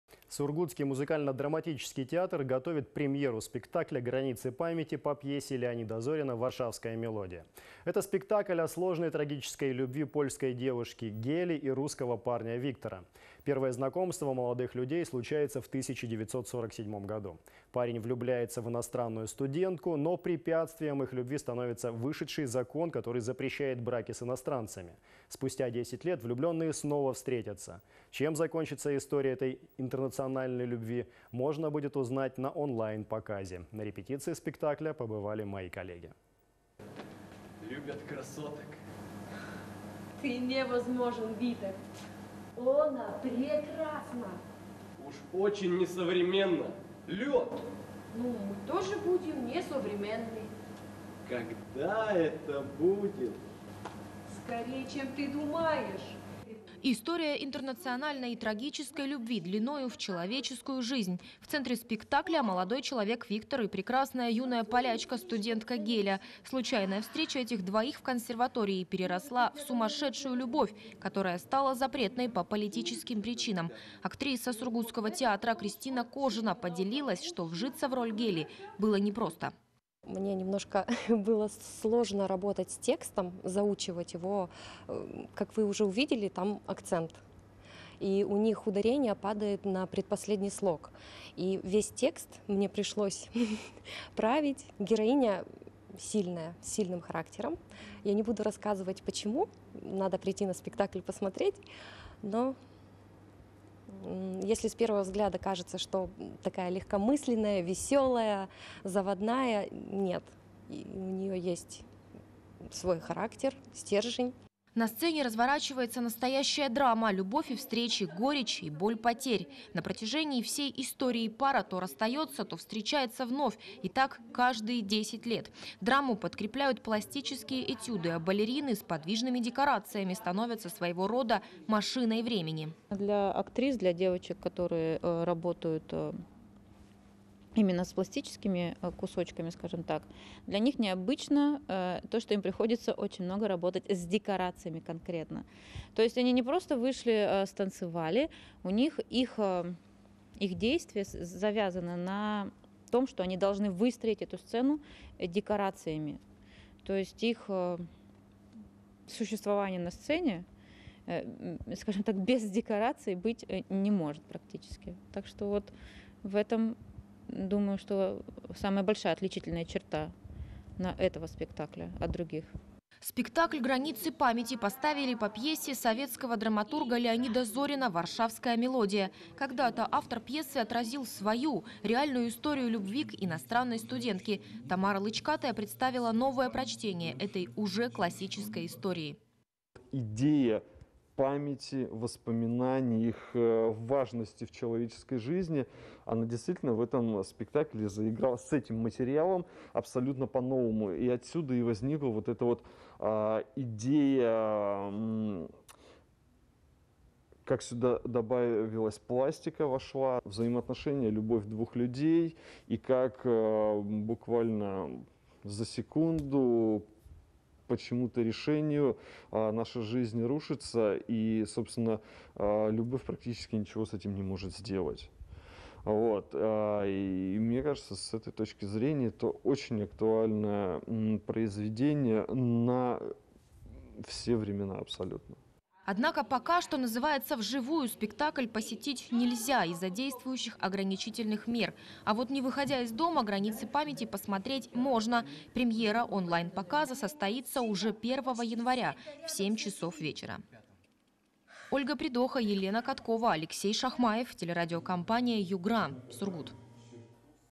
Сургутские театралы в первый день года покажут спектакль о трагической истории любви
Сургутский музыкально-драматический театр готовит премьеру спектакля «Границы памяти» по пьесе Леонида Зорина «Варшавская мелодия». Это спектакль о сложной, трагической любви польской девушки Гели и русского парня Виктора. Первое знакомство молодых людей случается в 1947 году. Парень влюбляется в иностранную студентку, но препятствием их любви становится вышедший закон, который запрещает браки с иностранцами. Спустя 10 лет влюблённые снова встретятся. Чем закончится история этой интернациональной любви можно будет узнать на онлайн-показе.